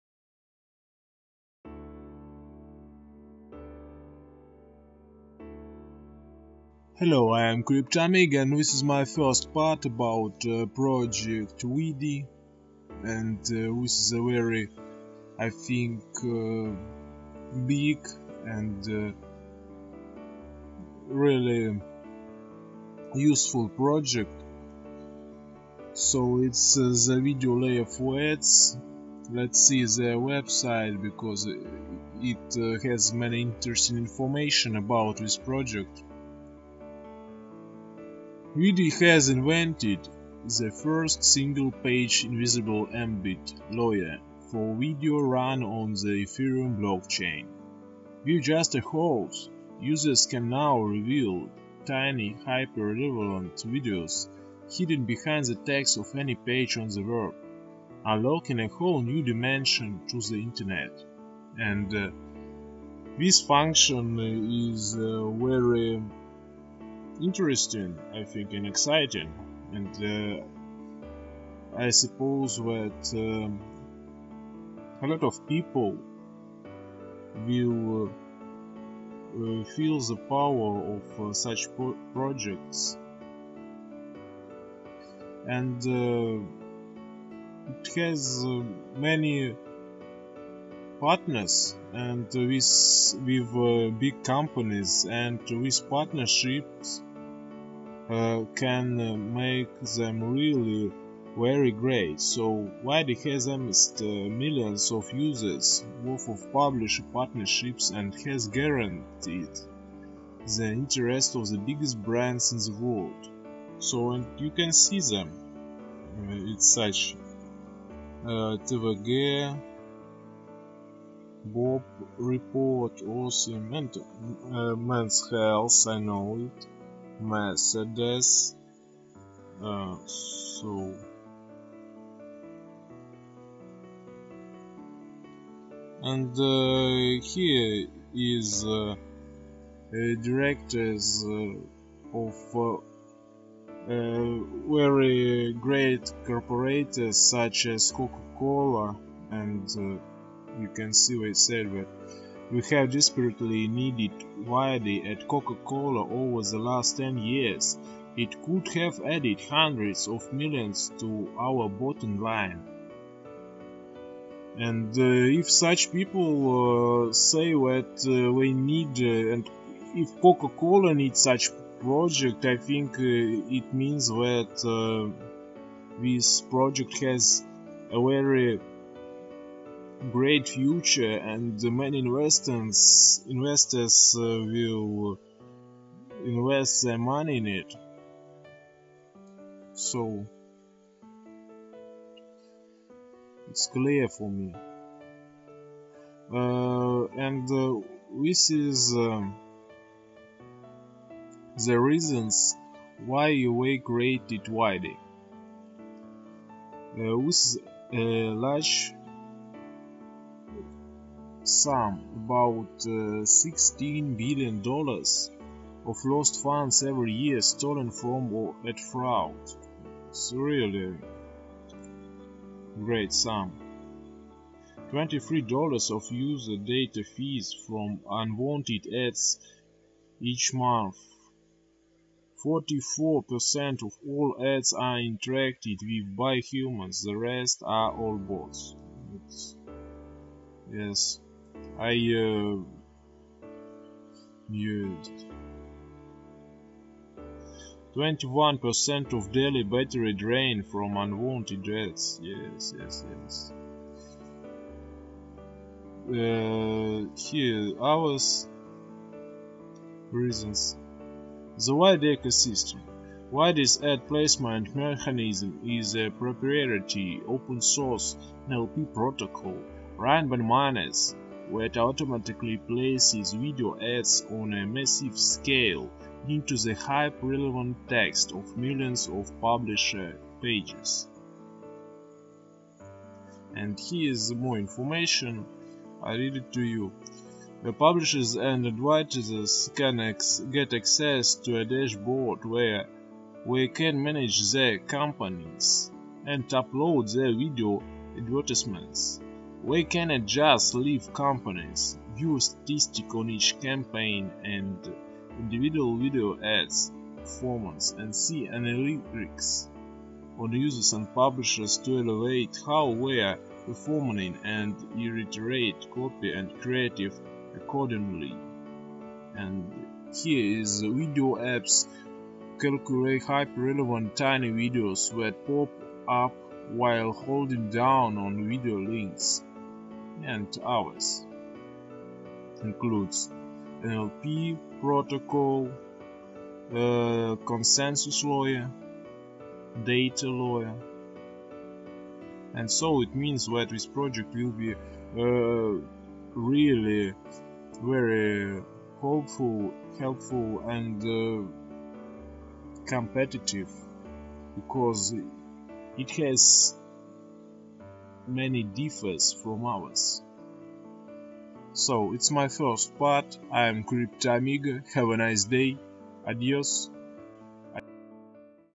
Part 1 VidyCoin - Decentralized Ad Network
Vidy has invented the first single-page invisible embed layer for video, run on the Ethereum blockchain. With just a hold, users can now reveal tiny hyper-relevant videos hidden behind the text of any page on the web, unlocking a whole new dimension to the internet.

wallet address - 0xD75894444bEa2f58d8fBb03536e9f5D91F4dDaDA